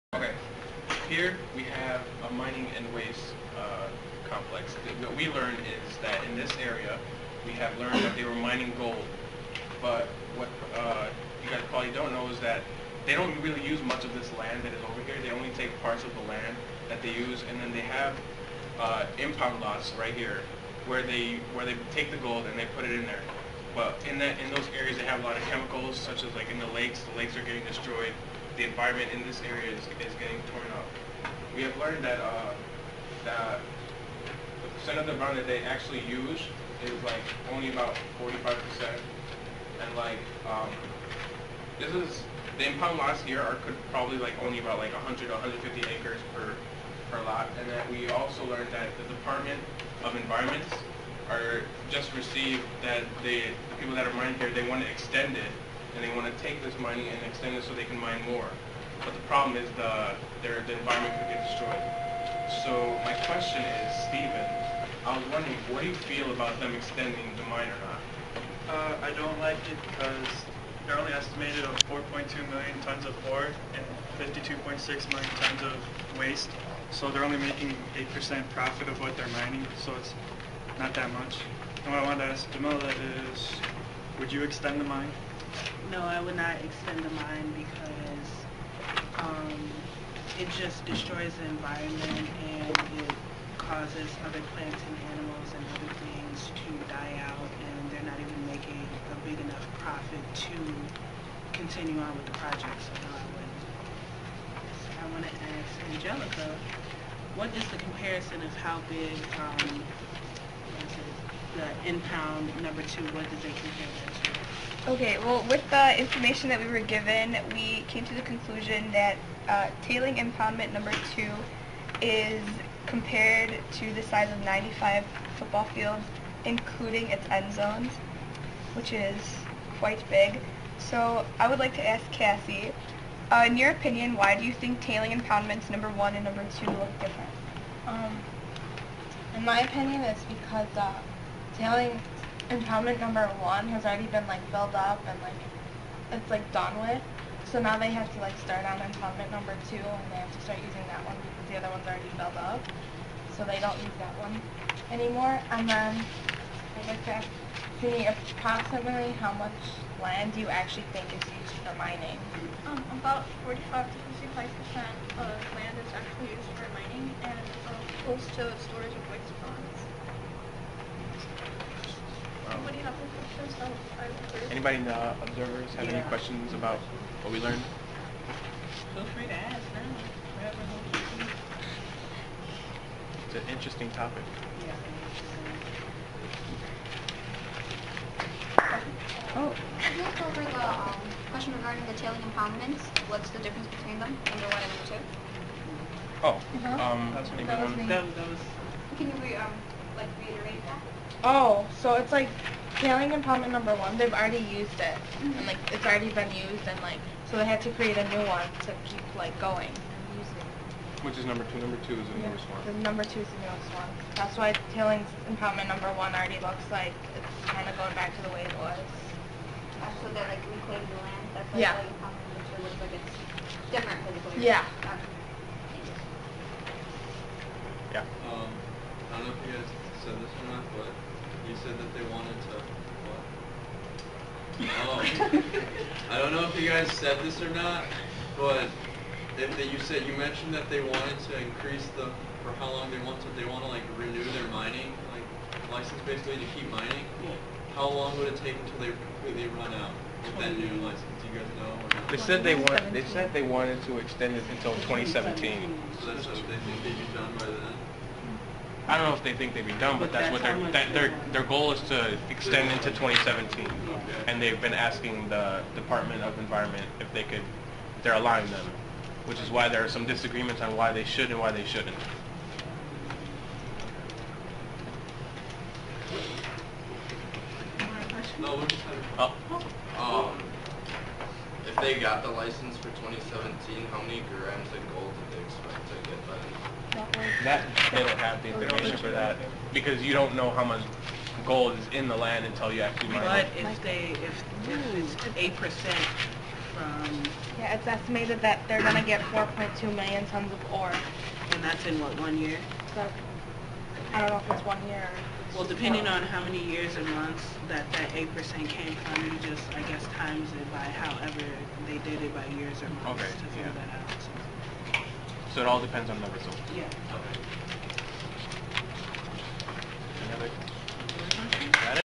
Geology students "fishbowl" discussion
Geology students are engaged in a "fishbowl" discussion format talking about mining in a United States gold mine.